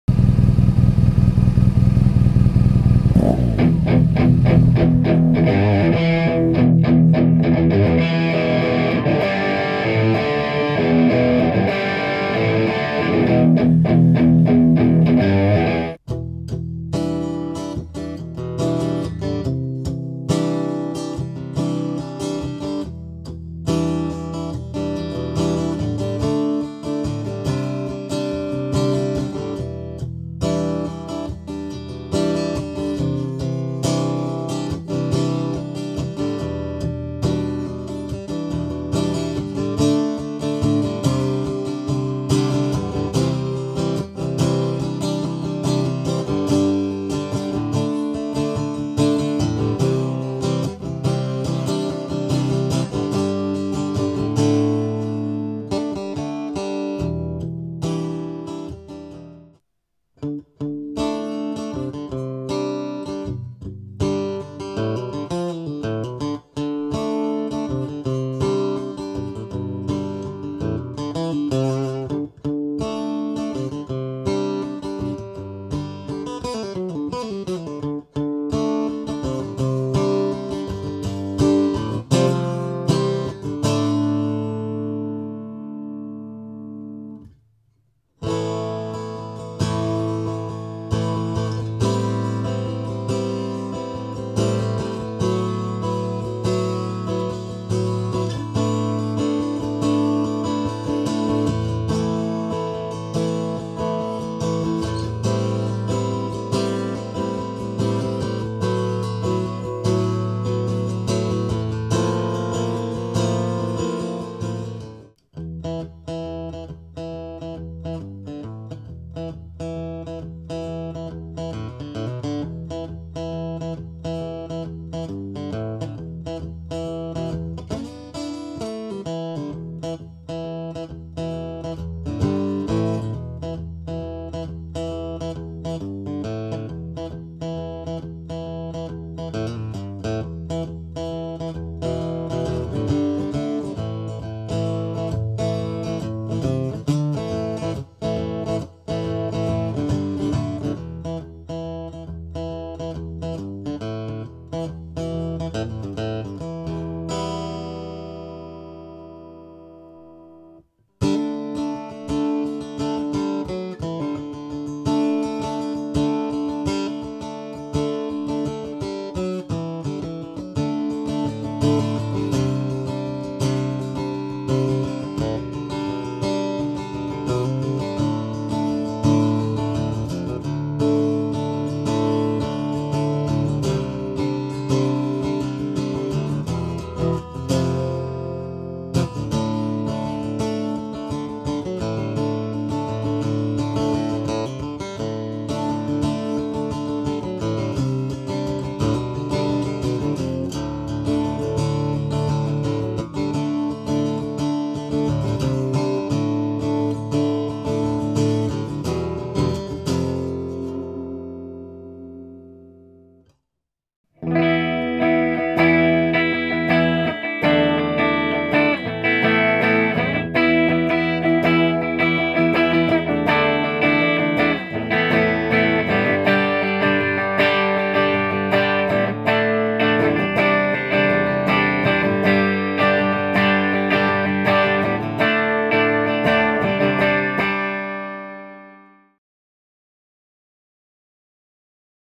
Anygig AGS(Steel Strings guitar) model Demo
Gig Anytime & Anywhere!!
Anygig AGS Steel strings Guitar model Demo video
Reviewed by Judge Fredd